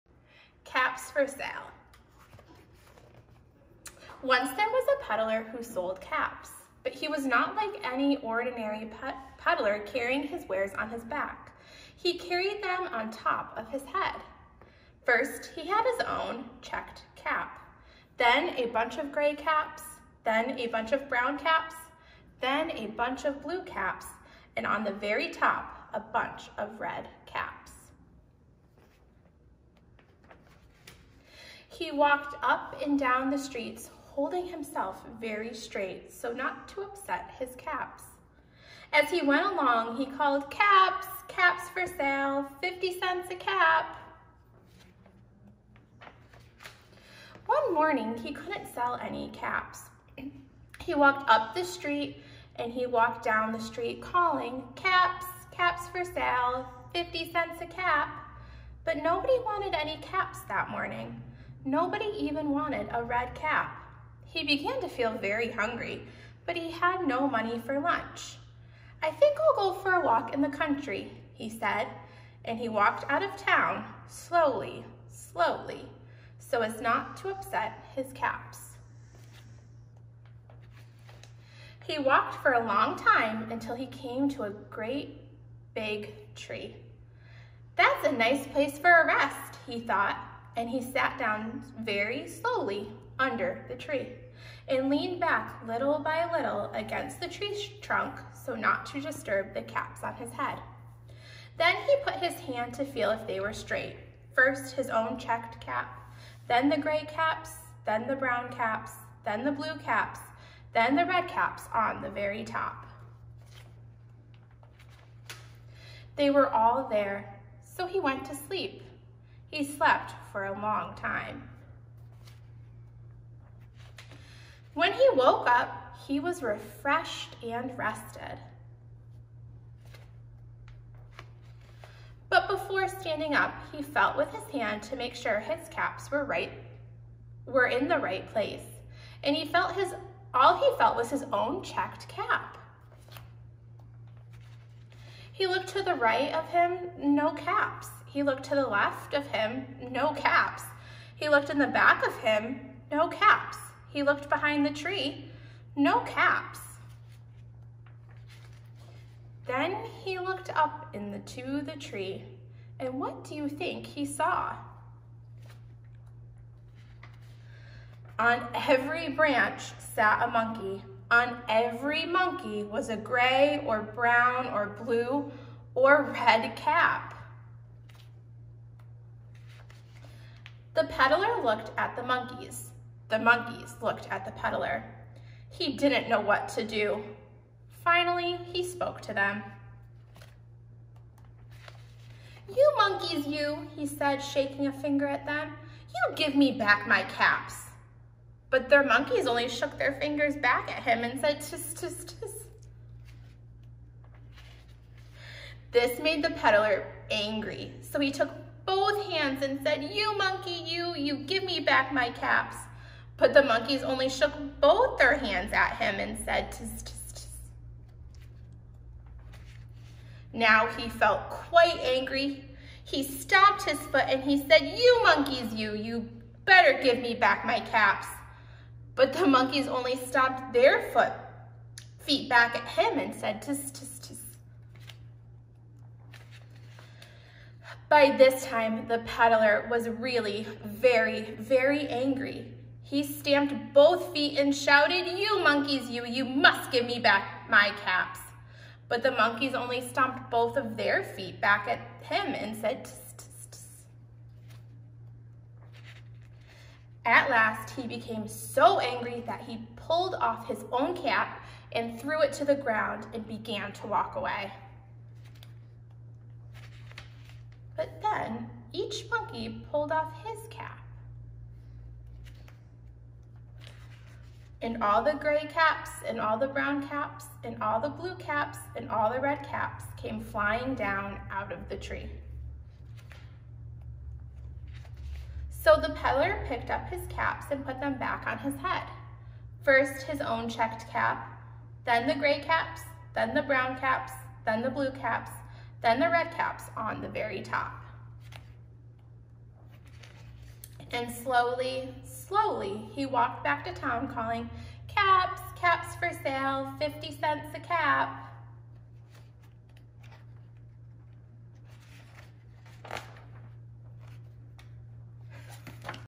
Caps for Sale by Esphyr Slobodkina
Caps for Sale by Esphyr Slobodkina read by Ms. DiFabio